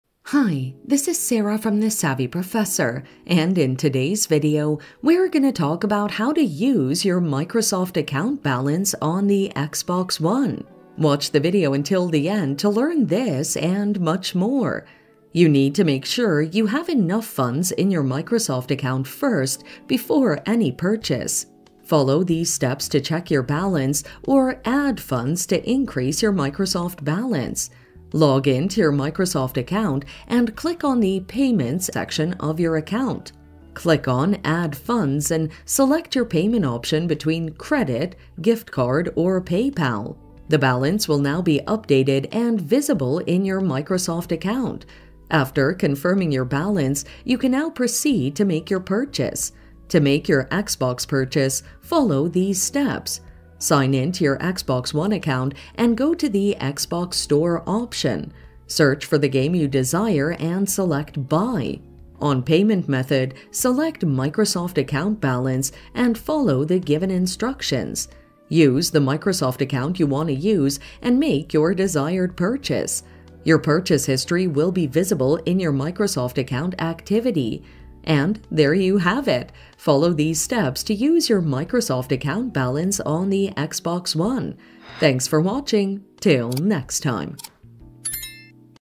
How To Use Microsoft Account Balance On Xbox One(How To Spend Microsoft Account Balance on Xbox One)
How To Use Microsoft Account Balance On Xbox One(How To Spend Microsoft Account Balance on Xbox One). In this video tutorial I will show you how to use Microsoft account balance on Xbox One.

You need to make sure you have enough funds in your Microsoft account first before any purchase. Follow these steps to check your balance or add funds to increase your Microsoft balance.
• Log in to your Microsoft account.
• Click on the “payments and billing” section of your account.
• Click on “Add funds" and select your payment option between credit, gift card or PayPal.
• The balance will now be updated and visible in your Microsoft account.

After confirming your balance, you can now proceed to make your purchase. To make your Xbox purchase, follow these steps.
• Sign in to your Xbox One account and go to the Xbox store option.
• Search for the game you desire and select “buy.”
• On payment method, select “Microsoft account balance” and follow the given instructions.
• Use the Microsoft account you want to use and make your desired purchase.
• Your purchase history will be visible in your Microsoft account activity.